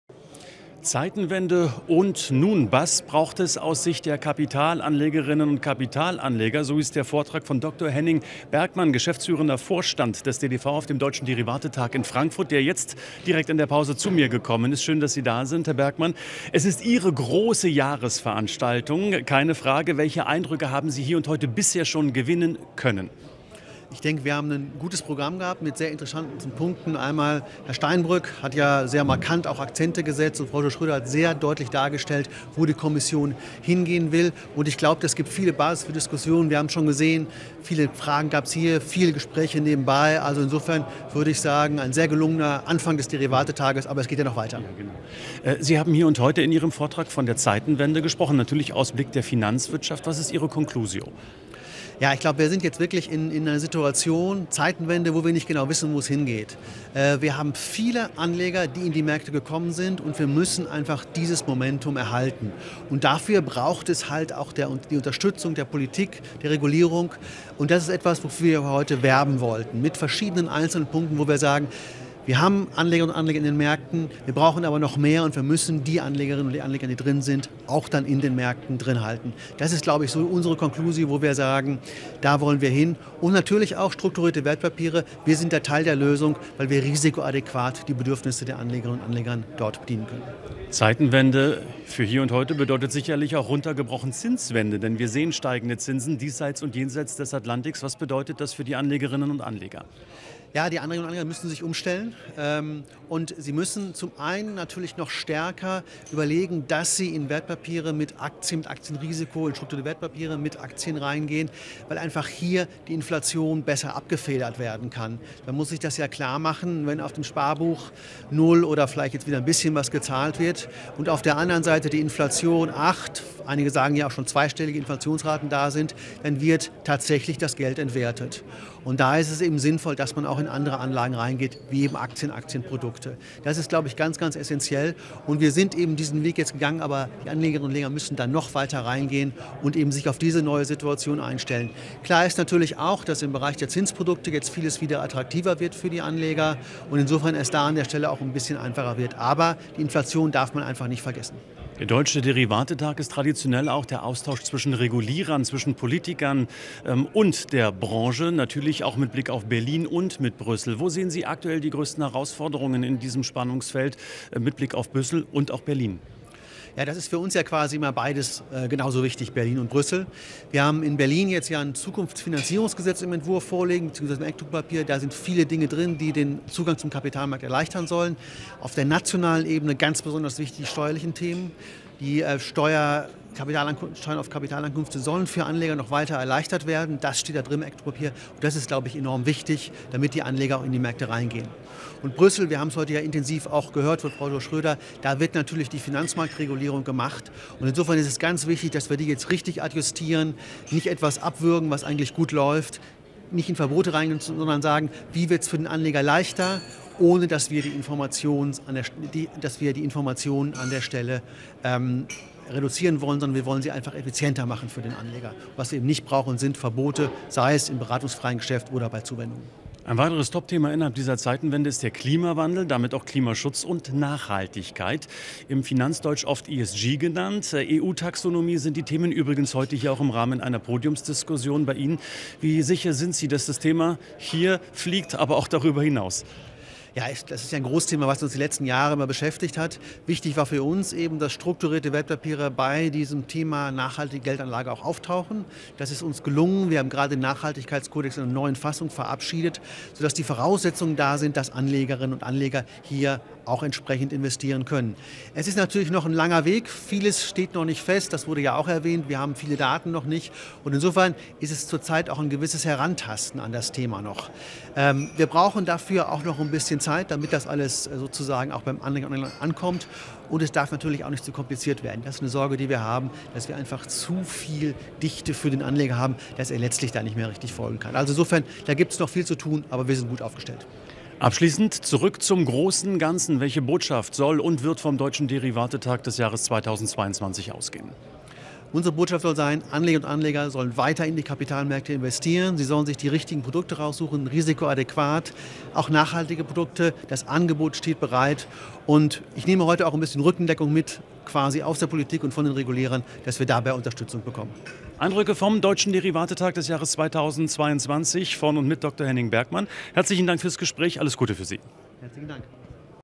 Deutscher Derivate Tag 2022: Im Zeichen der Zeitenwende - Interview mit Dr. Henning Bergmann, DDV
Dr. Henning Bergmann, geschäftsführender Vorstand des Deutschen Derivate Verbands (DDV), im Interview mit Andreas Franik beim Deutschen Derivate Tag 2022.